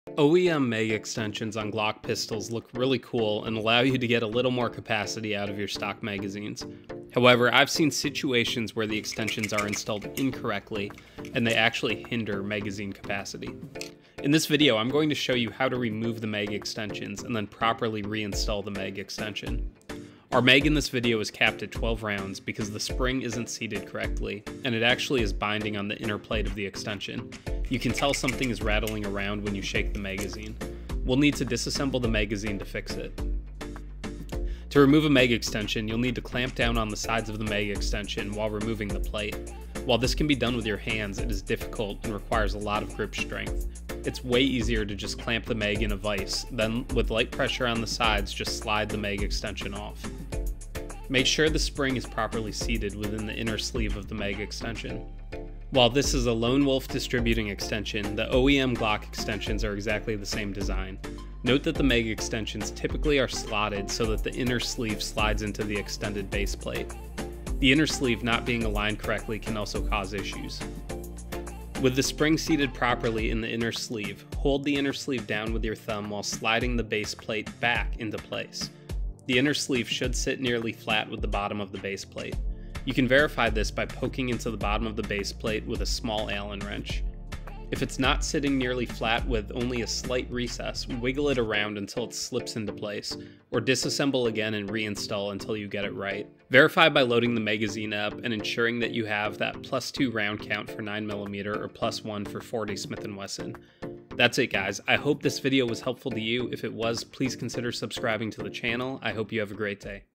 Install, Remove, or Fix Glock +2 Magazine Extensions
OEM Mag extensions on Glock pistols look really cool and allow you to get a little more capacity out of your stock magazines. However, I've seen situations where the extensions are installed incorrectly and they actually hinder magazine capacity.

In this video I'm going to show you how to remove the mag extensions and then properly re-install the mag extension.

Our mag in this video is capped at 12 rounds because the spring isn't seated correctly and is actually binding on the inner plate of the extension. You can tell something is rattling around when you shake the magazine. We'll need to disassmble the magazine to fix it.

To remove a mag extension, you'll need to clamp down on the sides of the mag extension while removing the plate. While this can be done with your hands, it is difficult and requires a lot of grip strength. Its way easier to just clamp the mag in a vice with light pressure on the sides and just slide the mag extension off.

Make sure that the spring is properly seated within the inner sleeve of the mag extension. While this is a Lone wolf distributing extension, the OEM glock extensions are exactly the same design. Note that the mag extensions typically are slotted so that the inner sleeve slides into the extended base plate. The inner sleeve not being aligned correctly can also cause issues.

With the spring seated properly in the inner sleave, hold the inner sleave down with your thumb while sliding the base plate back into place. The inner sleave should sit nearly flat with the bottom of the base plate. You can verify this by poking into the bottom of the baseplate with a small allen wrench. If its not sitting nearly flat with only a slight recess, wiggle it around until it slips into place, or disassemble again and reinstall until you get it right.

Verify by loading the magazine up and ensuring you have that +2 round count for 9mm or +1 for 40 S&W.

That's it, I hope this video was helpful to you. If was please consider subbing to the channel, I hope you have a great day.